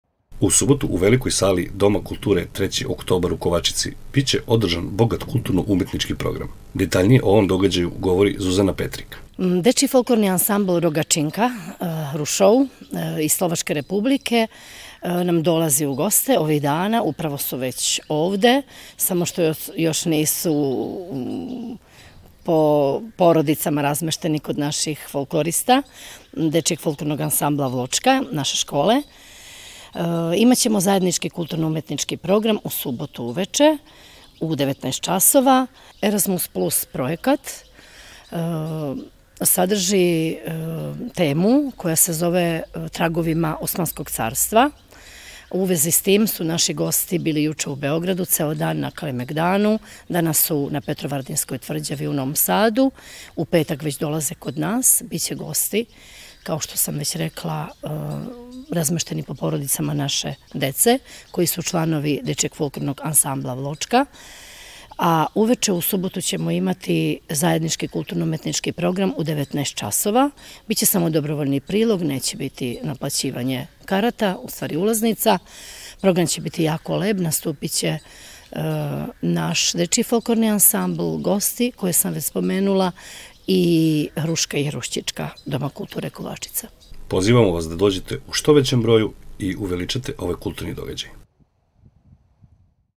Folklorni spektakl sutra u Kovačici
U domu kulture „3. oktobar“ u Kovačici u subotu 24. juna biće održan bogat kulturno umetnički program u kom će učestvovati dečji folklorni ansambli iz Kovačice i njihovi gosti iz Slovačke republike. Program počinje u 19 sati.